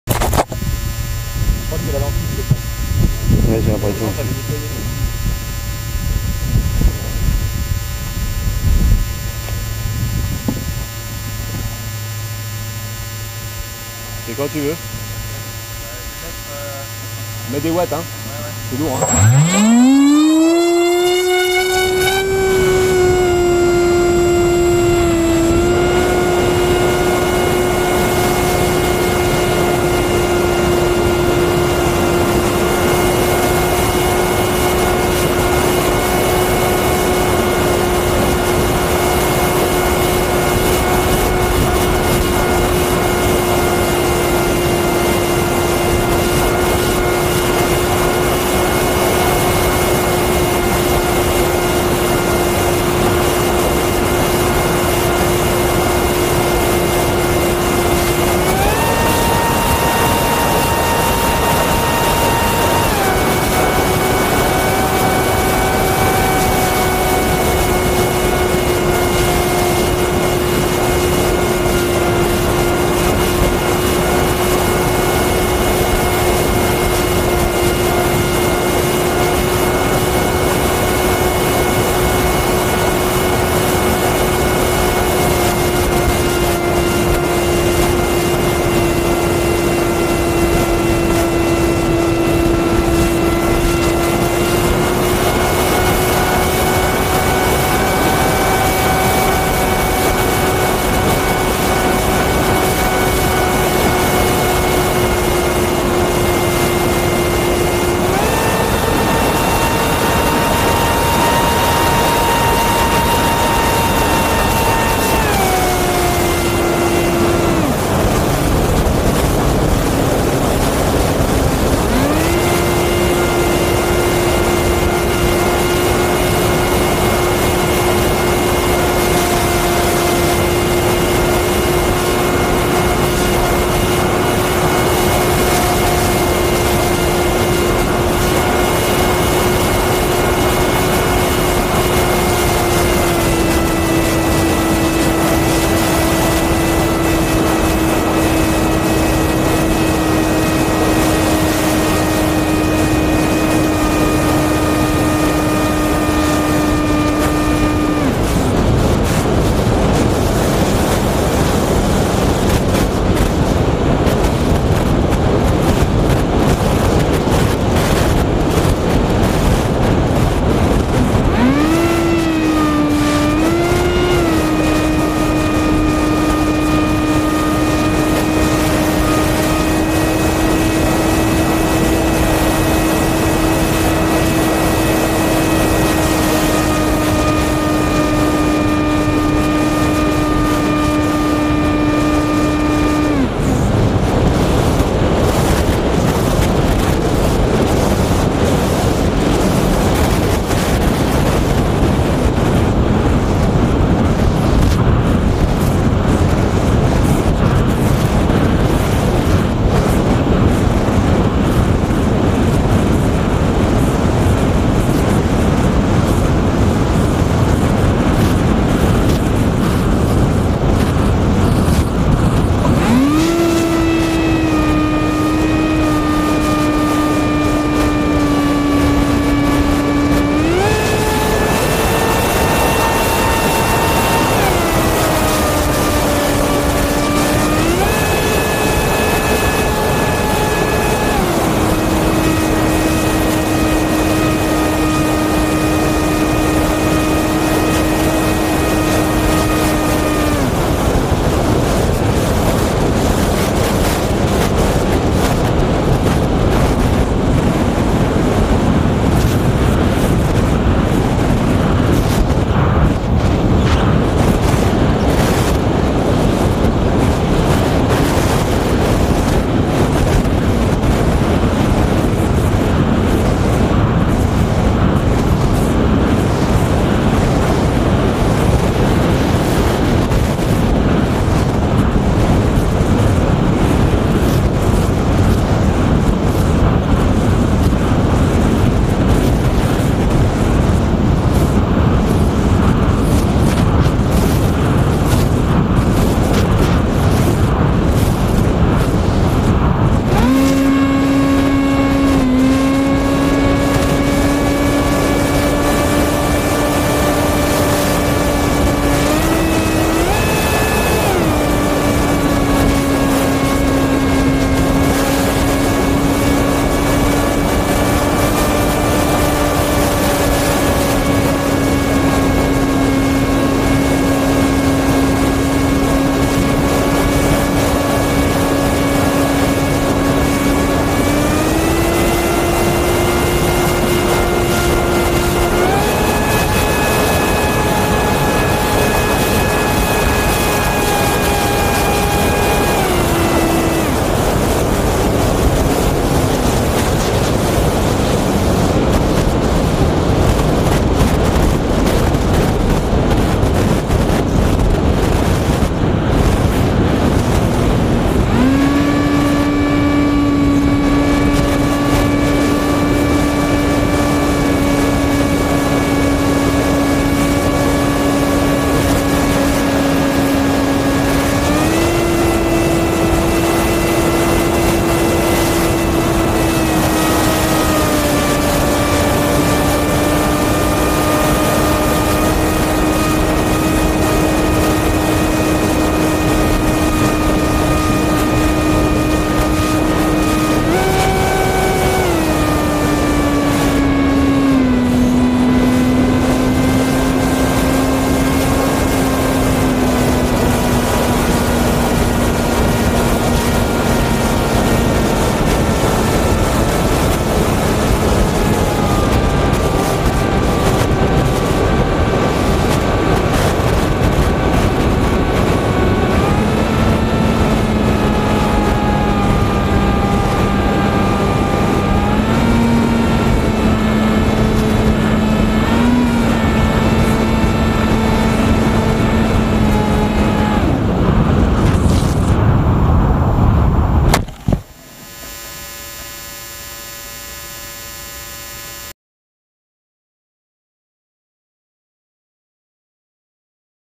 Maiden MiniRaceWing
Built in two evenings ! This is the second maiden of this wing. The first one goes wrong, I set the CG to 135mm as required with a 1800mAh 4S lipo but it was too tail heavy, the wing crashed in 3 parts as intended. I repaired it in 5min: unscrew, assemble wings to the frame, screw and connect servo wires and it's done.

With this maiden I used a heavier 2200mAh 4S to be front heavy (at 135mm) and it was much better, perfect!
Landing was hard and broke a wing, again quickly repaired.

Setup:
F80 2500kv
ESC Cobra 60A
Prop HQ6045
4S 2200mAh Optipower
TBS Crossfire
Camera HS1177
TBS Unify Pro 5.8 V2